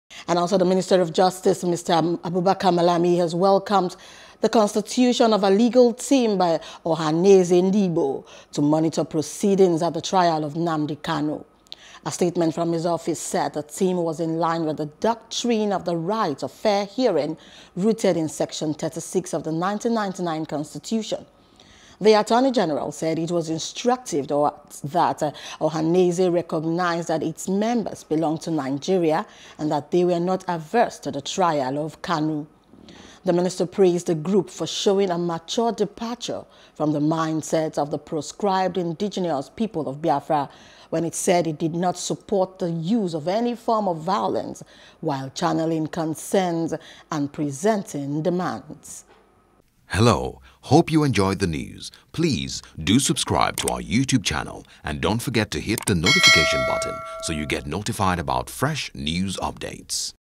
FG Welcomes Ohanaeze Legal Team | NEWS
The Minister of Justice, Mr Abubakar Malami, has welcomed the constitution of a legal team by Ohanaeze Ndigbo to monitor proceedings at the trial of Nnamdi Kanu.